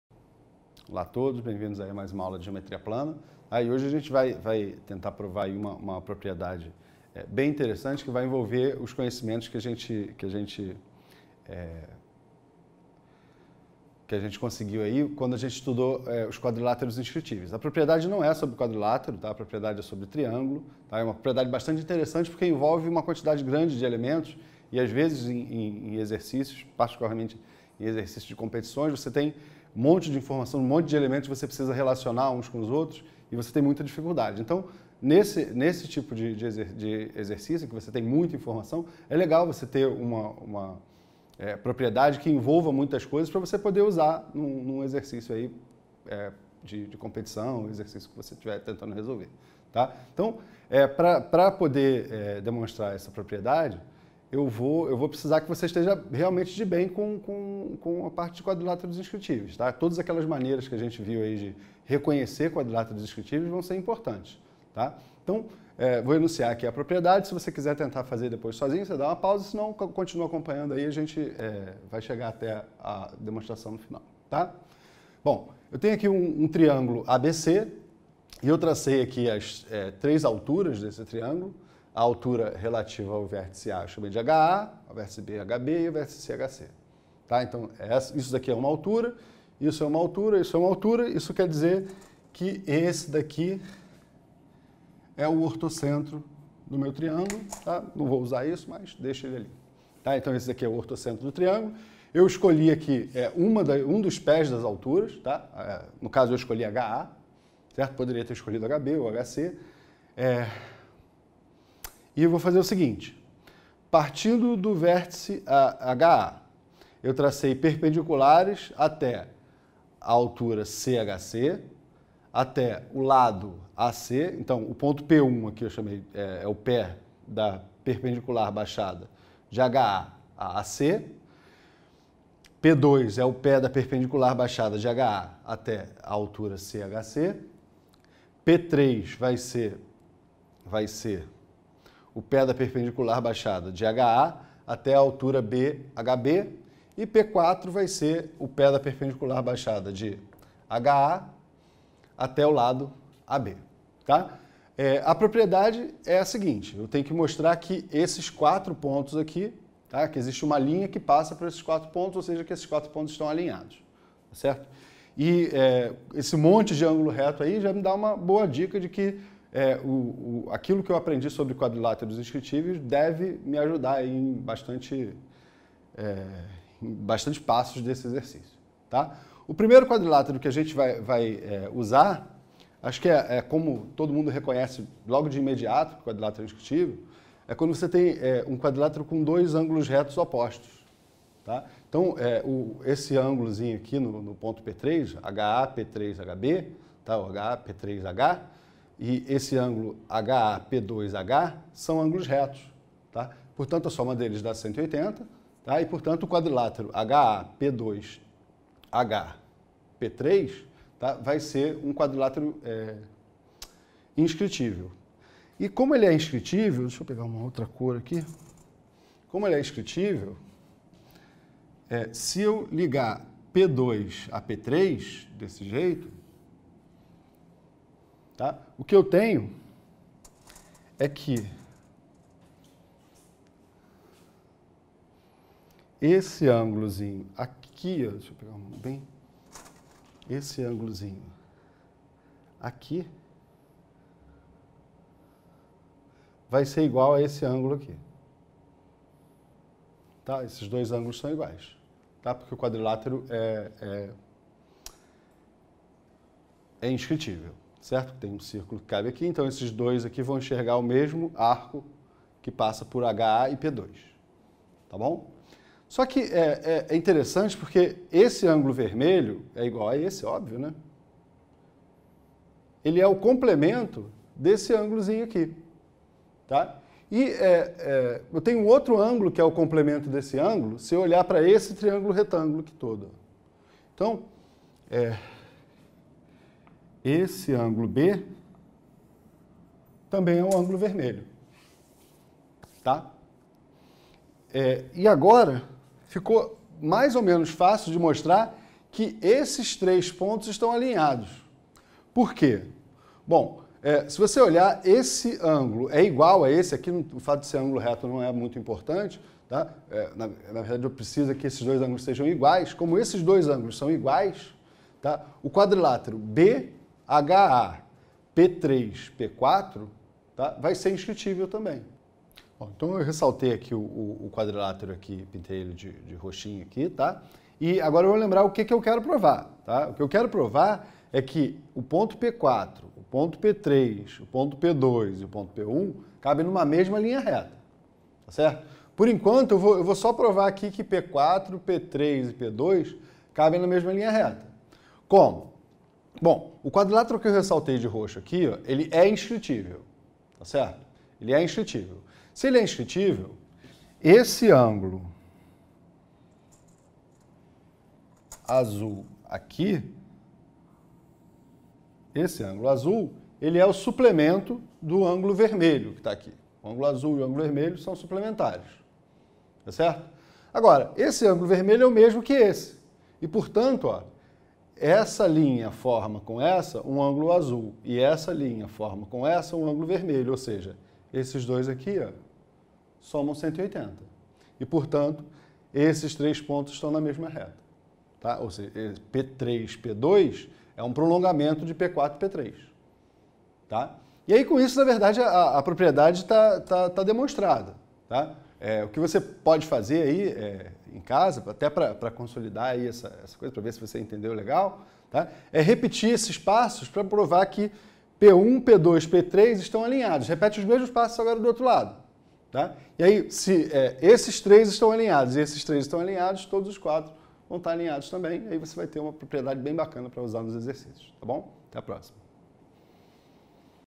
Geometria - Aula 47 - Exercício sobre quadriláteros inscritíveis
Professor Marcos Paulo Ferreira de Araújo

Nesta aula apresentamos um problema cuja solução é baseada na identificação e uso de propriedades de quadriláteros inscritíveis.